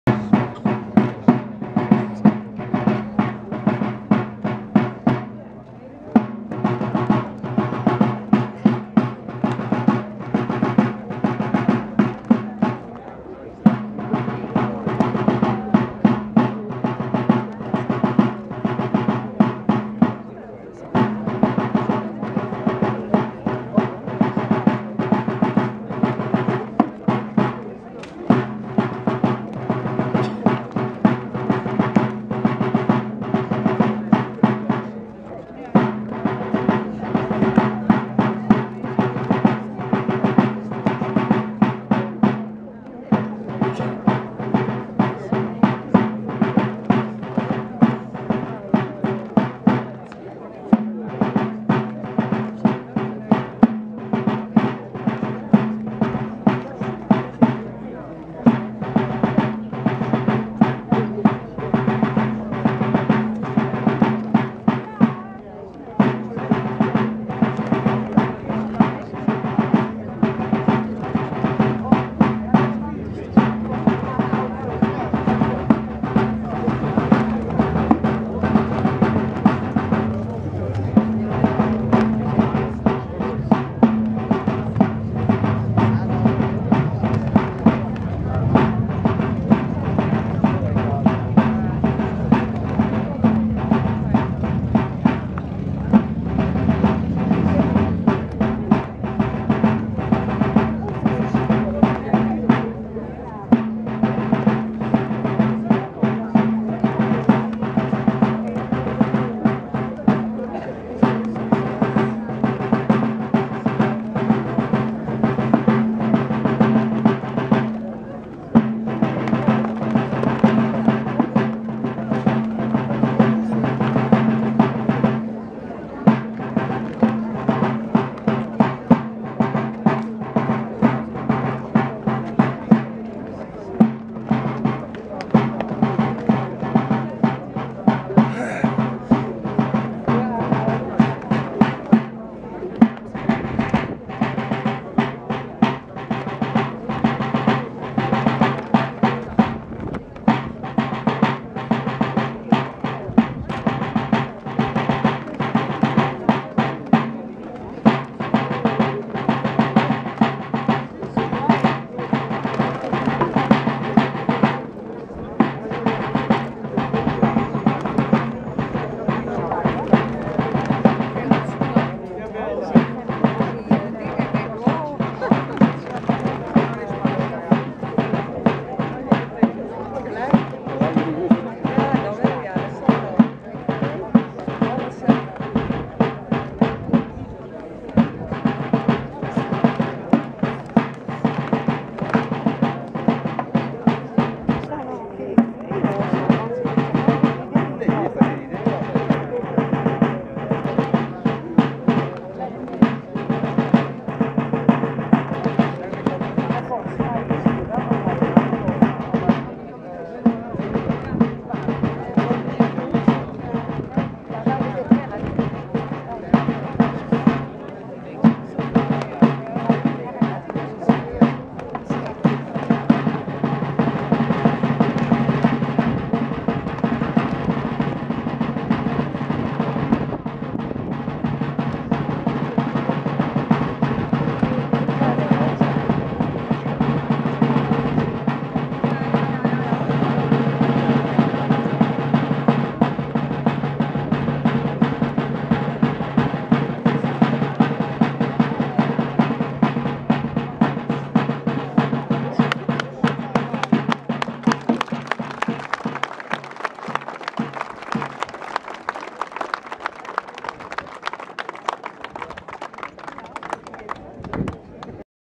Vendelgroet Gilde rosmalen voor Jan Renders tbv 25 jaar diaken
Het gilde bracht Diaken Jan Renders zondag 27 mei een vendelgroet bij de viering van 25 jaar diaken